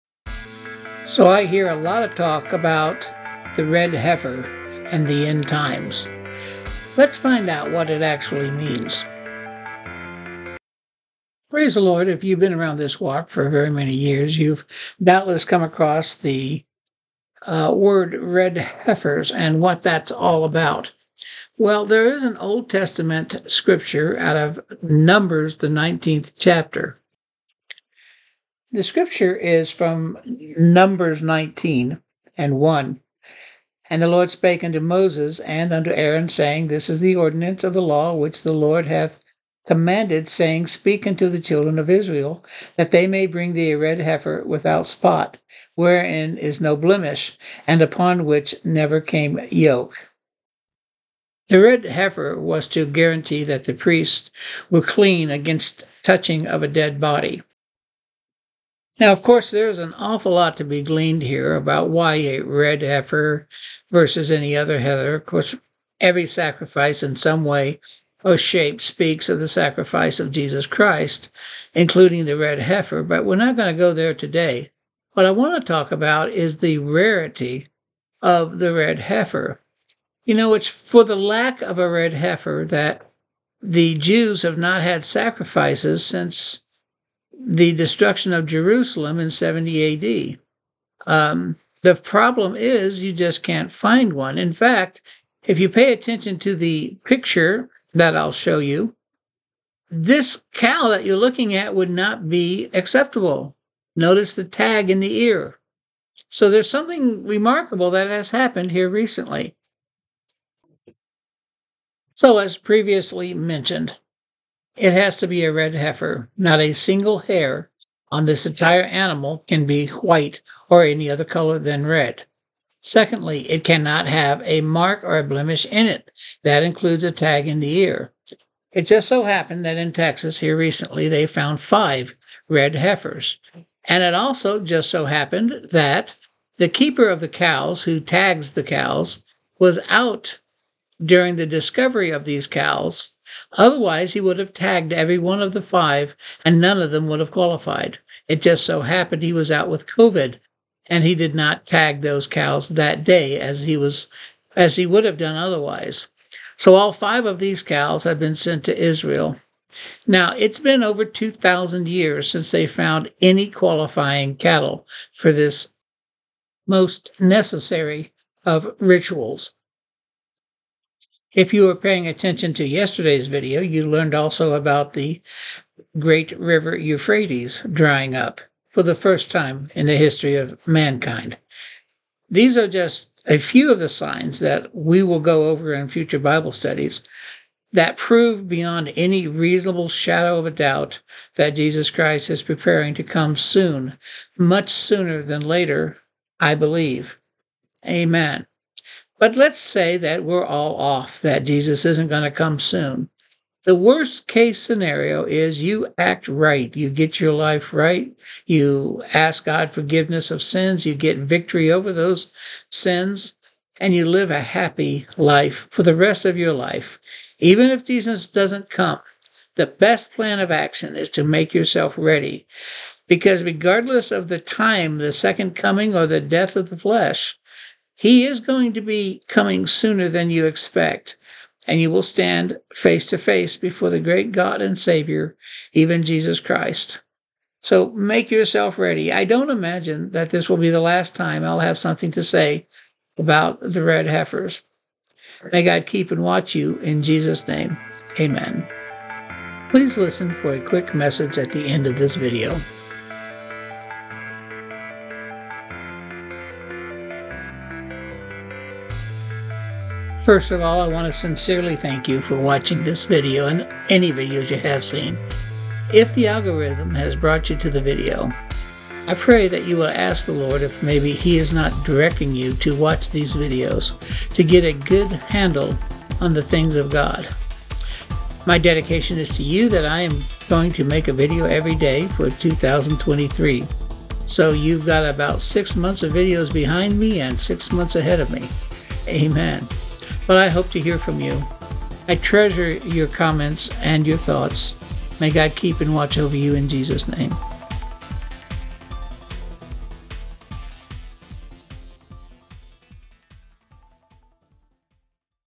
June 24 2023 The Red Heifer
Welcome to daily bible study for you, this page is designed to offer a short bible study every day for your commute to work or over your morning coffee or routine!
This bible study touches the Red Heifer as seen in Numbers 19